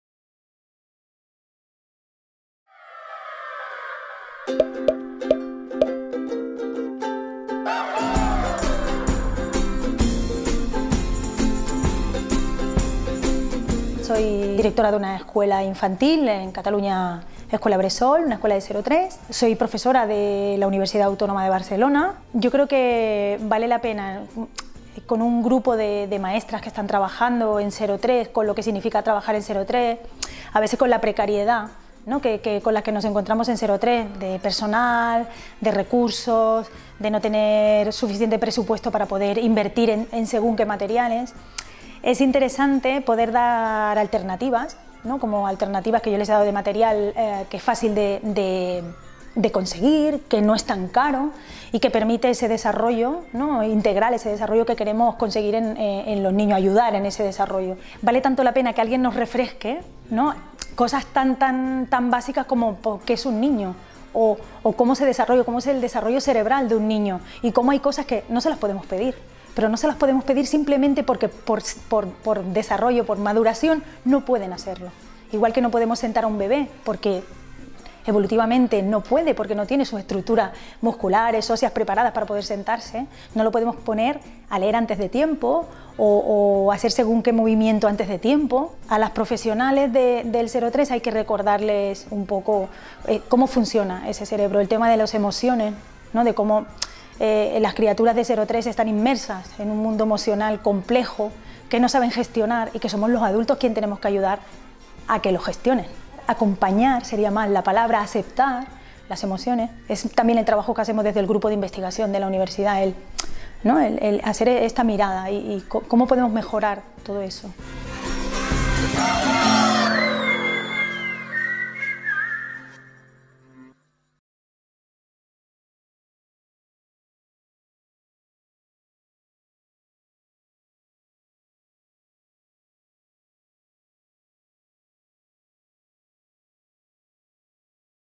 Mar Pérez - Jornada educación infantil 0-3 años
Mar Pérez, maestra de educación infantil y profesora de la Universidad Autónoma de Barcelona, ponente en las Jornadas de Educación Infantil 0-3 años nos da si visión de aspectos básicos del desarrollo psicoevolutivo del alumnado de estas edades.